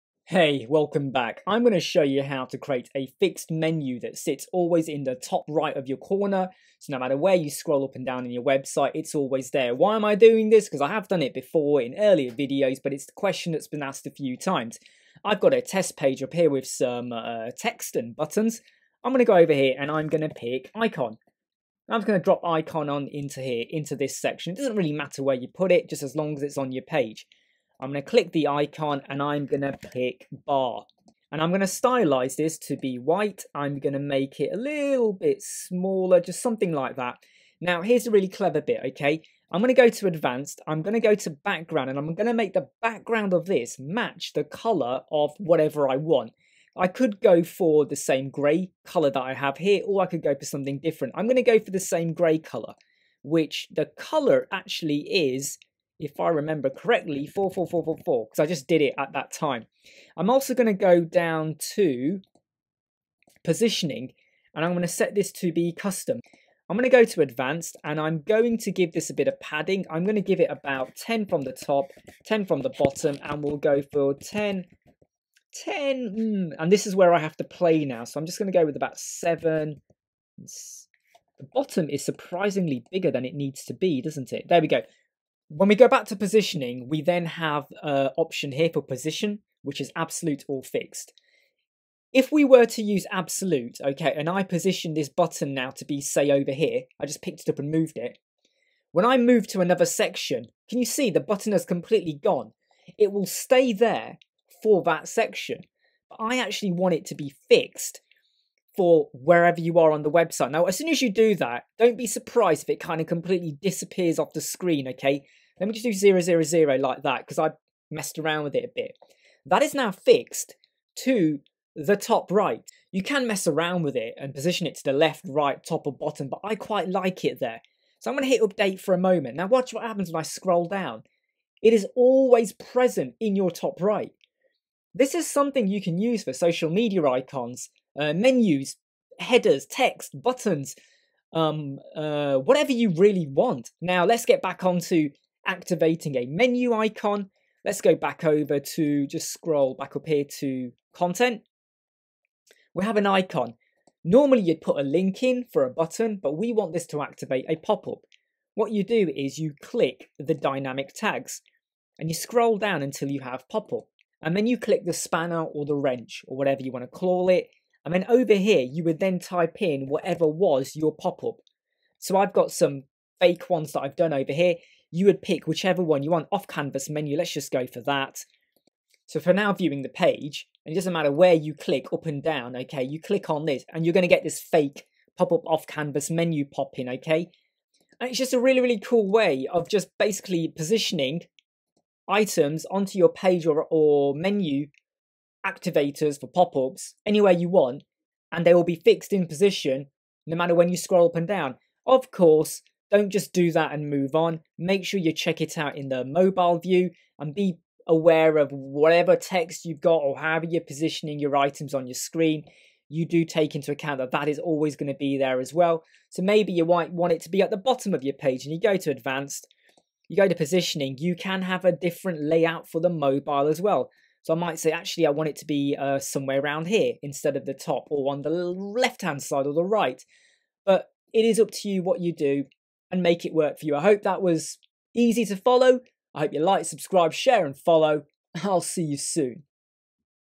Elementor - Use Fixed Icons to Activate an Off-Canvas Menu
Often gets asked so let's look at making an icon fixed on the screen that activates an Off-Canvas menu.